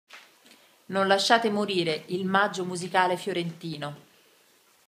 Micaela Carosi: Non lasciate morire il Maggio Musicale Fiorentino
L'appello di Micaela Carosi, soprano, in solidarietà con il Teatro del Maggio Musicale Fiorentino
FIRMATE LA PETIZIONE! SIGN THE PETITION!